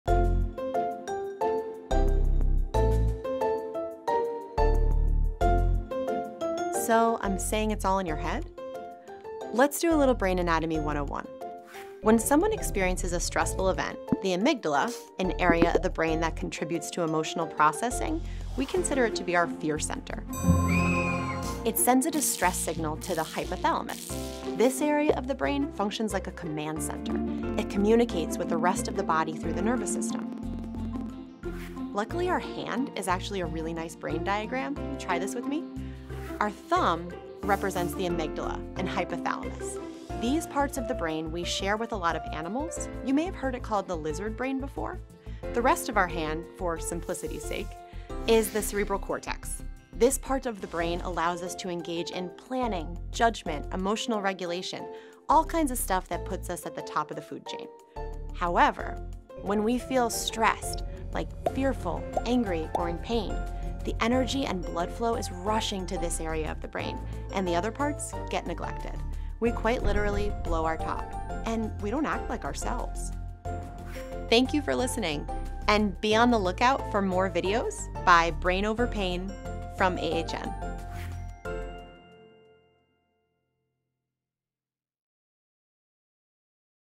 Pain Psych FAQs- "So you're saying it's all in my head?"
When we know more about our bodies, we are better at taking care of them. Mental health awareness also means understanding how our thoughts impact our physical health.